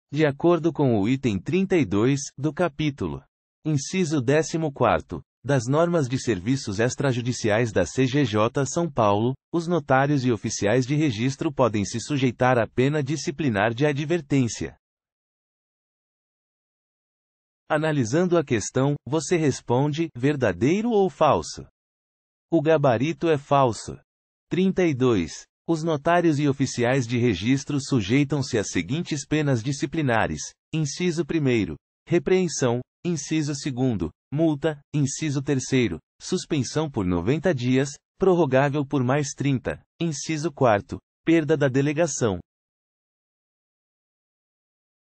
QScon Direito: [Item 32, Cap. XIV, NSCGJSP] Decorando a Lei Seca
Item 32, Cap. XIV, NSCGJSP | Decorando a Lei Seca

De acordo com o item 32, do Capítulo. inciso décimo quarto: das Normas de Serviços Extrajudiciais. da CGJ-SP, os notários e oficiais de registro podem se sujeitar à pena. disciplinar de advertência.

Gabarito: Falso